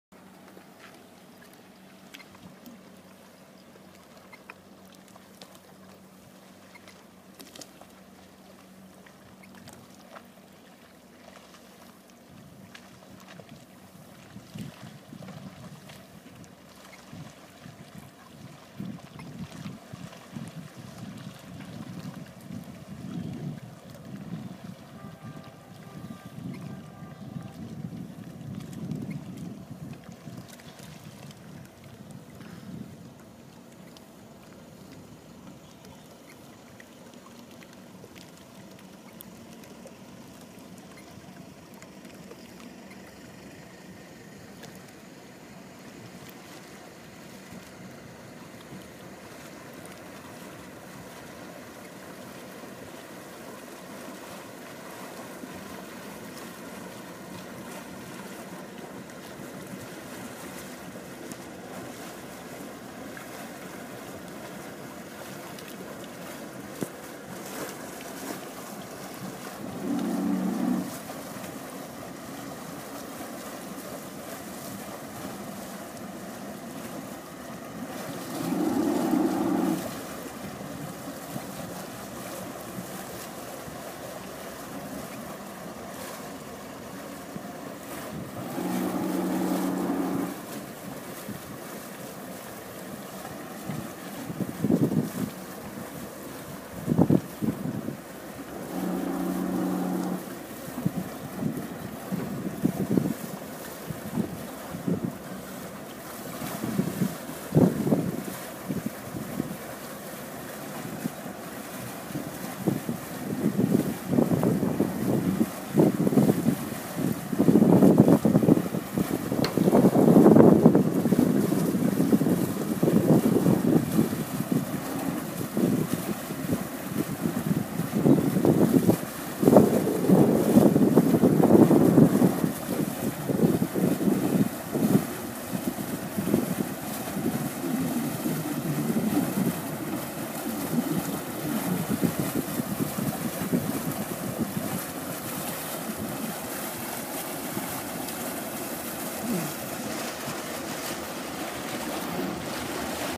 Barefootin Sealine sc35
Docking Barefootin in mayflower marina